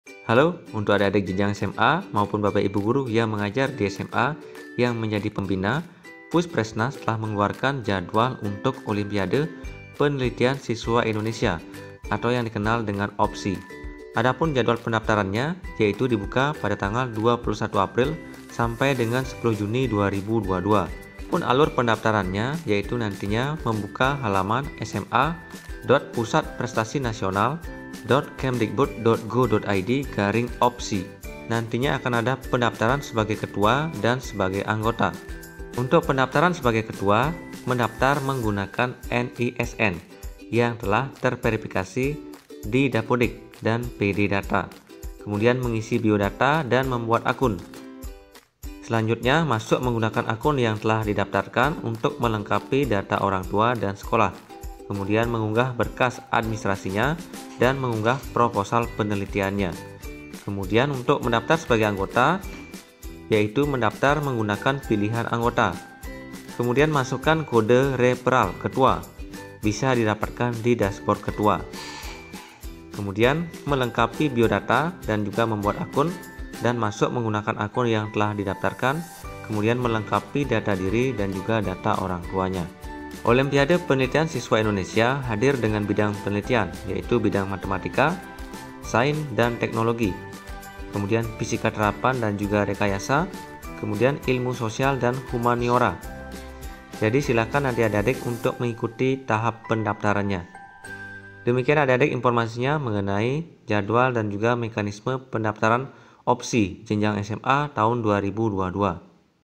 Jadwal dan Cara Daftar Olimpiade Penelitian Siswa Indonesia (OPSI) Jenjang SMA 2022 Puspresnas
Olimpiade Penelitian Siswa Indonesia (OPSI) SMA/MA hadir dengan bidang penelitian Matematika, Sains, dan Teknologi (MST), Fisika Terapan dan Rekayasa (FTR), dan Ilmu Sosial dan Humaniora (ISH). Dijamin semua bidangnya seru dan bisa mengasah kemampuan menelitimu!

Nah SobatPrestasi, siapa aja nih yang udah nggak sabar untuk mendaftar di OPSI 2022 SMA/MA?